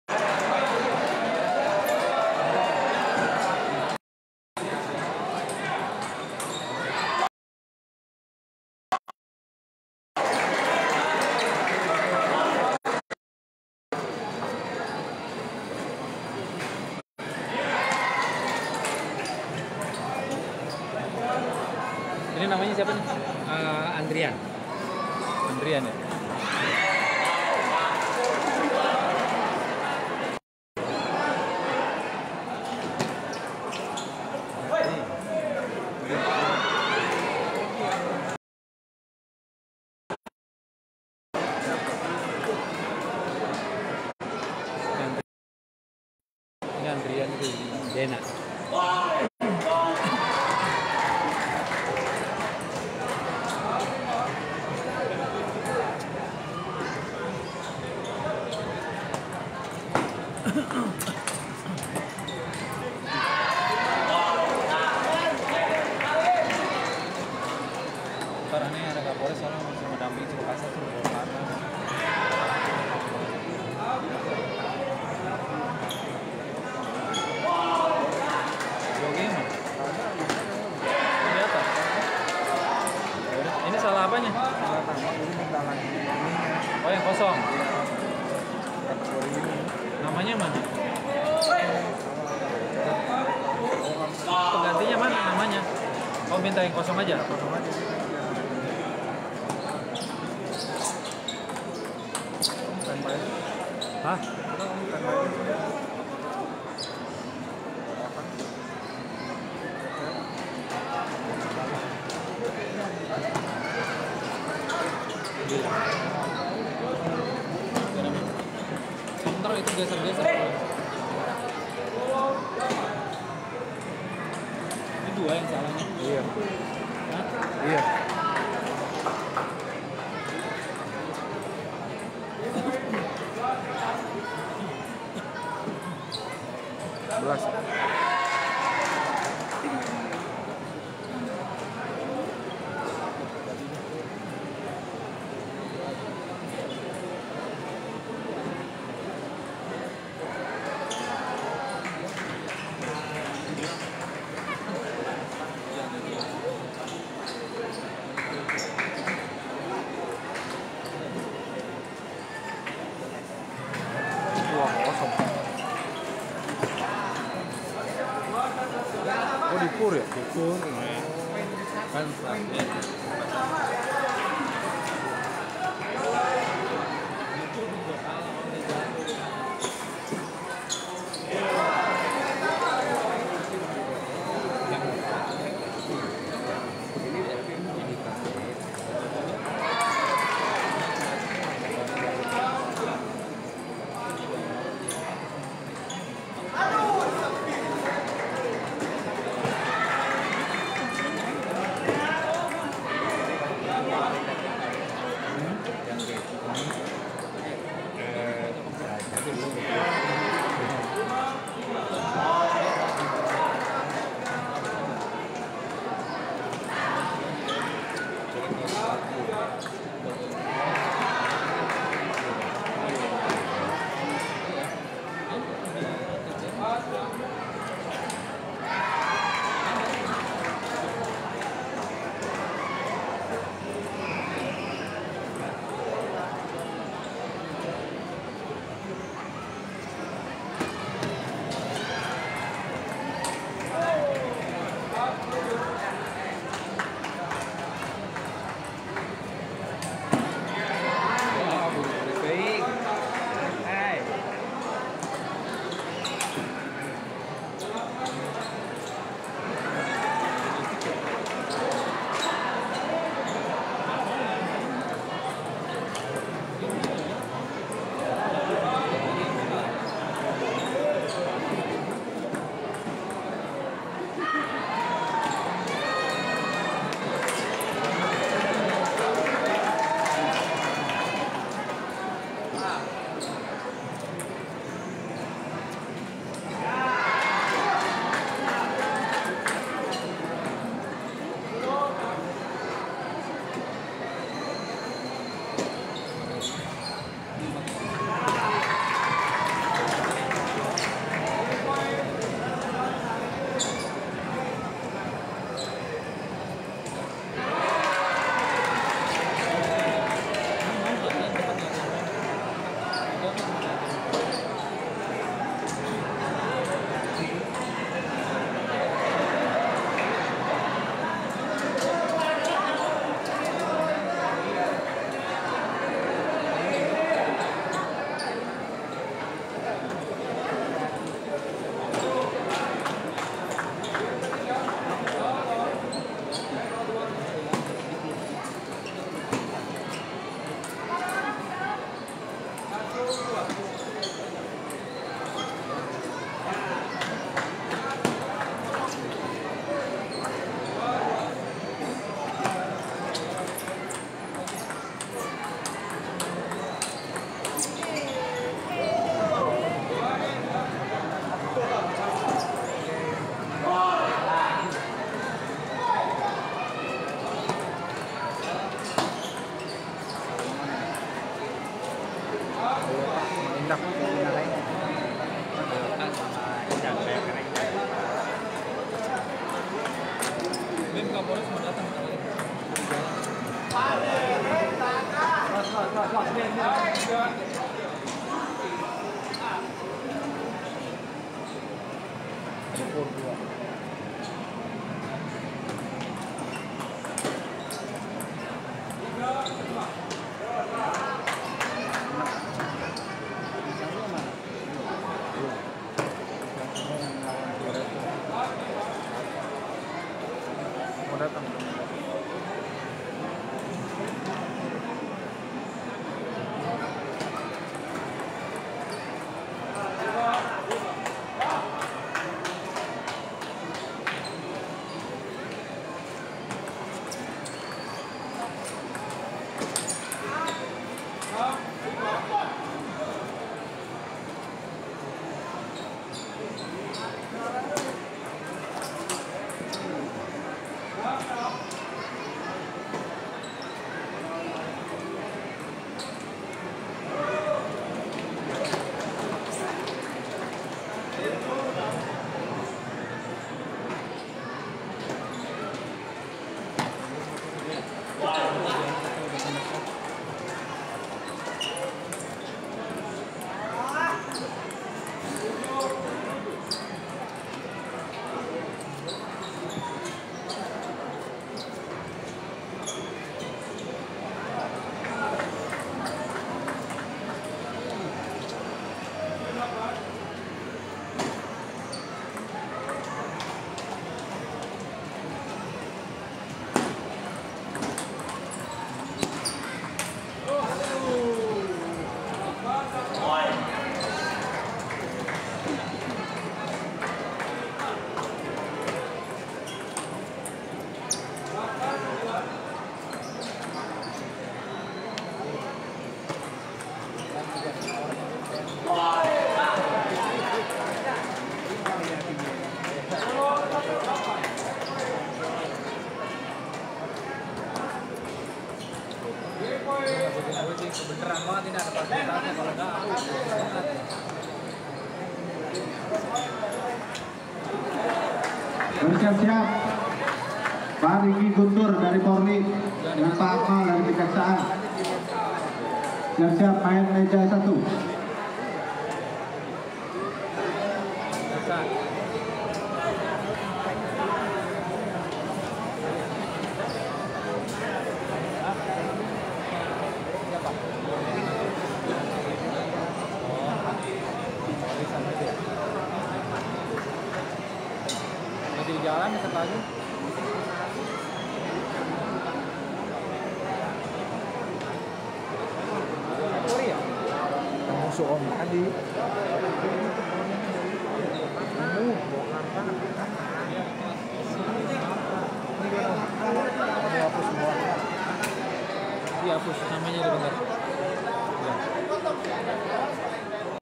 Turnamen Tenis Meja Final Kelas Umum Kapolres Cup Andrian Toyota Melawan Dena Indag
Turnamen Tenis Meja Final Kelas Umum (A) Kapolres Cup Andrian Toyota Melawan Dena Indag